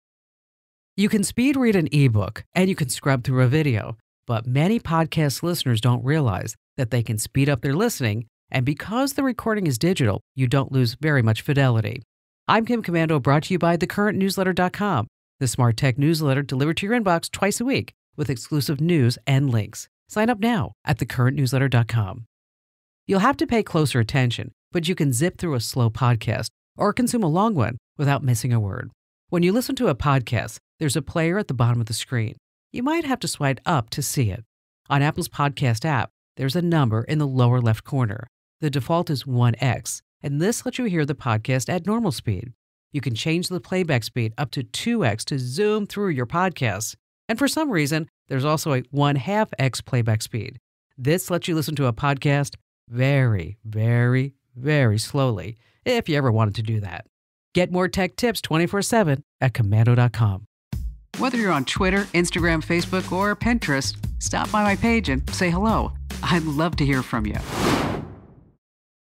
How to listen to podcasts more quickly
Want to spend less time listening to podcasts? Listen now to speed up your shows. (I love this tech trick because you don't lose much fidelity. Plus, you're saving a ton of time!)